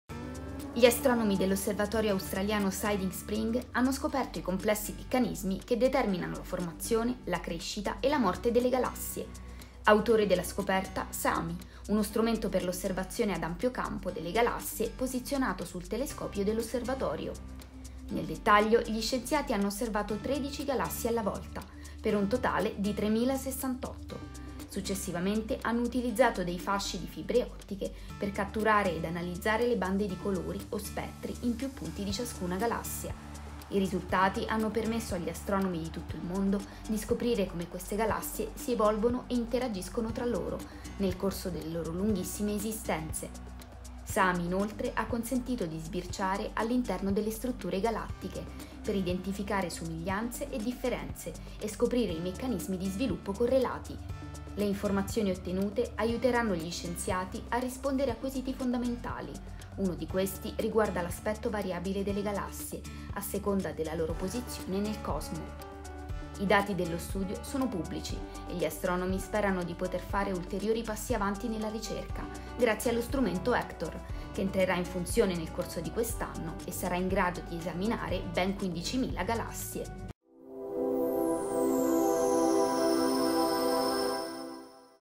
Sami svela i dettagli di oltre 3000 galassie
Lo strumento dell'osservatorio australiano di Side Spring ha svelato i dettagli di oltre 3000 galassie

Gli astronomi dell’osservatorio australiano Siding Spring hanno determinato i complessi meccanismi che determinano la formazione, la crescita e la morte delle galassie. Autore della scoperta, lo strumento Sami - uno strumento per l’osservazione ad ampio campo delle galassie posizionato sul telescopio dell’osservatorio. Nel dettaglio gli scienziati hanno osservato 13 galassie alla volta, per un totale di 3068. Successivamente hanno utilizzato dei fasci di fibre ottiche per catturare e analizzare bande di colori, o spettri, in più punti di ciascuna galassia.

I risultati hanno permesso agli astronomi di tutto il mondo di esplorare come queste galassie evolvono e interagiscono tra loro nel corso delle loro lunghissime esistenze. Sami inoltre ha consentito di sbirciare all’interno delle strutture galattiche per identificare somiglianze e differenze e scoprire i meccanismi di sviluppo correlati. Le informazioni ottenute aiuteranno gli scienziati a rispondere a quesiti fondamentali, uno di questi riguarda l’aspetto variabile delle galassie a seconda della loro posizione nel cosmo. I dati dello studio sono pubblici e gli astronomi sperano di poter fare ulteriori passi avanti nella ricerca grazie allo strumento Hector che entrerà in funzione nel corso di quest’anno e sarà in grado di esaminare ben 15mila galassie.